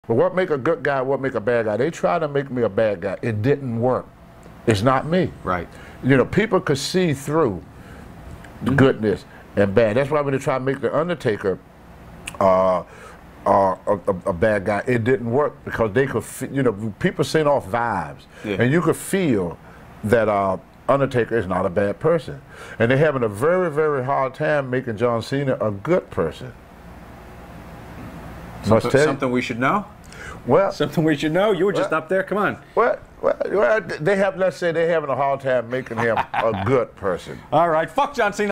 Tony Atlas shoots on John Cena
Anthony White talks about John Cena.